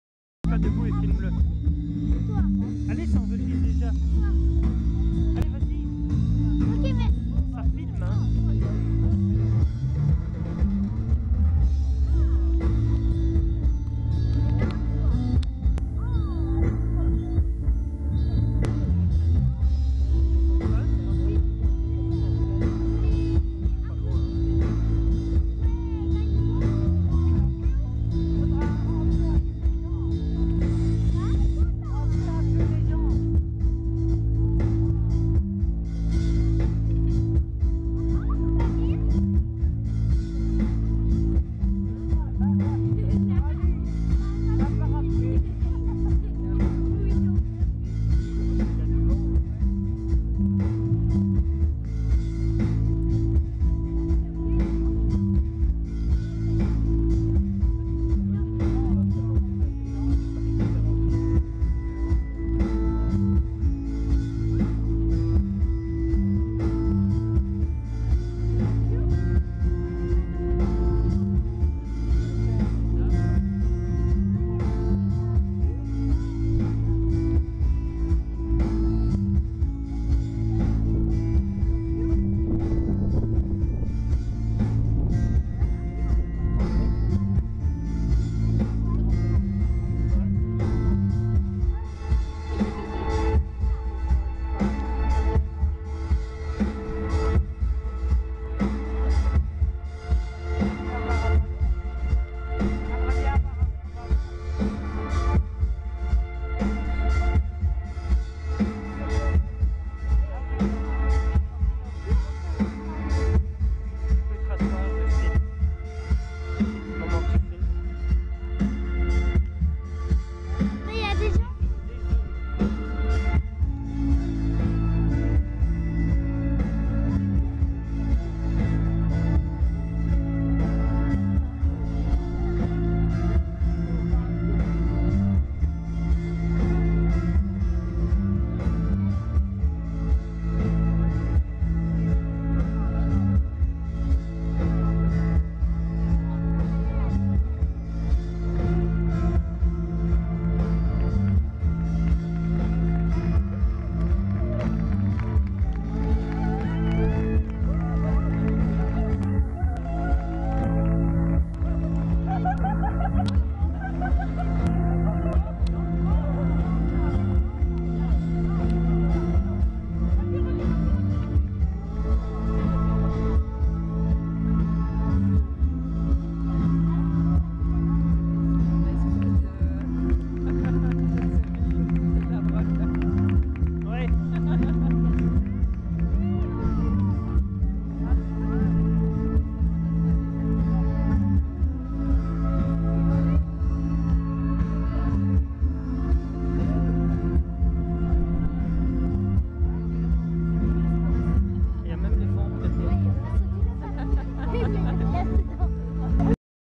Le géant
Spectacle final des 50 ans du centre reine Fabiola à Neufvilles